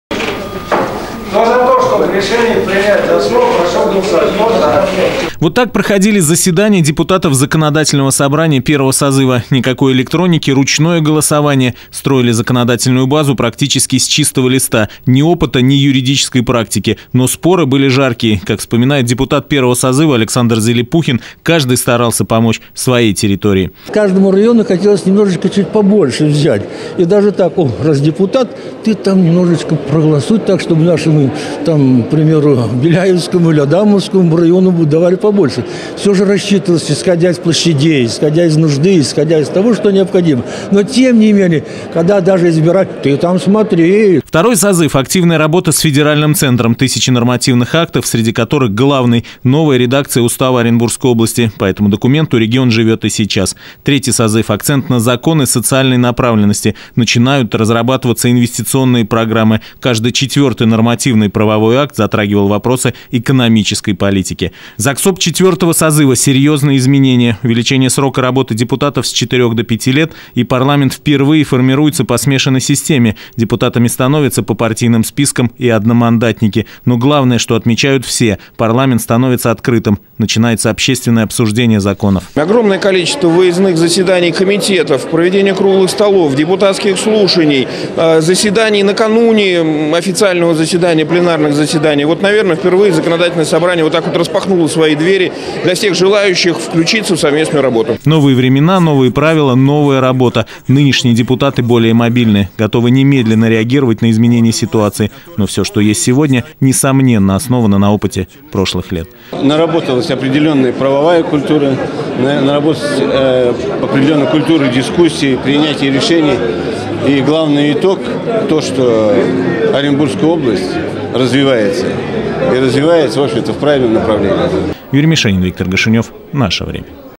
Заксобу 20 27 04 14
20 лет законотворчества. Областное Законодательное Собрание  отмечает юбилей. Как всё начиналось вспоминали накануне на торжественном собрании. На встречу пришли депутаты всех созывов. На встрече побывал и наш корреспондент Юрий Мишенин, небольшой исторический экскурс в его сюжете. Кстати некоторые кадры о работе областных парламентариев абсолютно эксклюзивные.